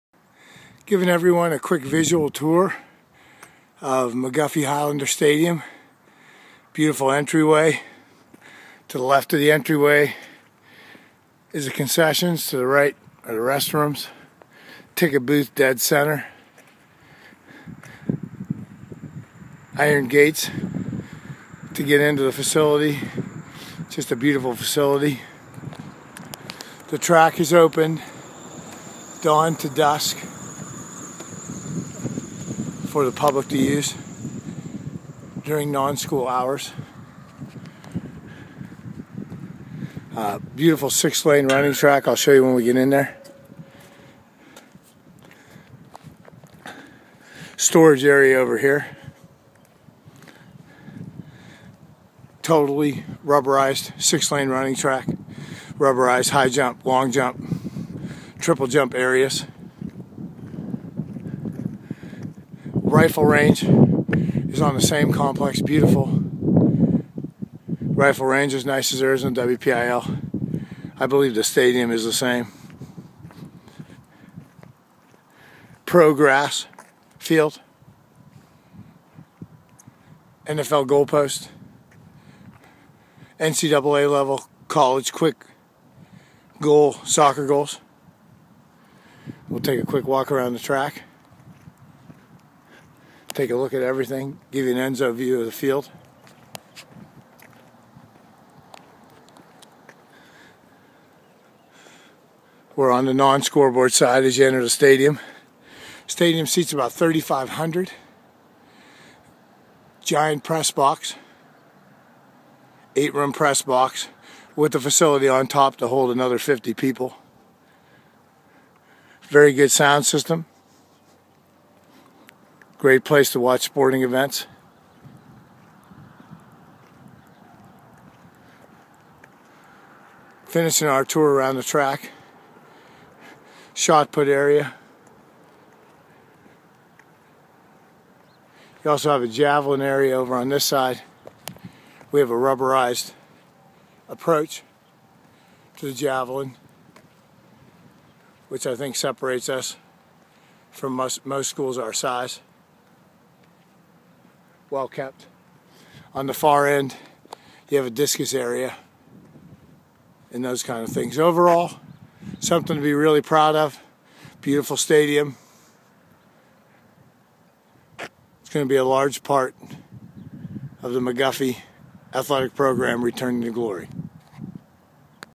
McGuffey Highlander Stadium Tour
A walking tour of McGuffey Highlander Stadium